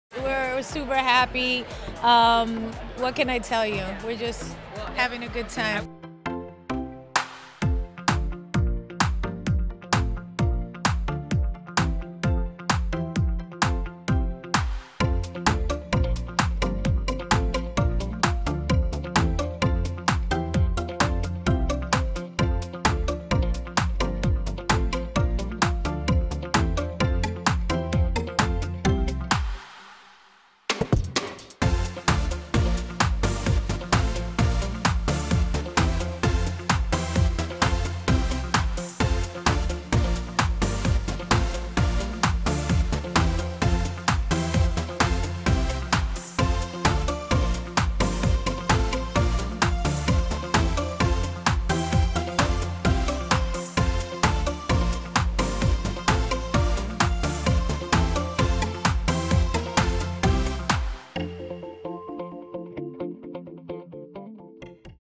Jennifer Lopez and Ben Affleck won’t last, her first husband says
Jennifer Lopez’s first husband has provided details as proof of why he believes she will end up divorcing Ben Affleck.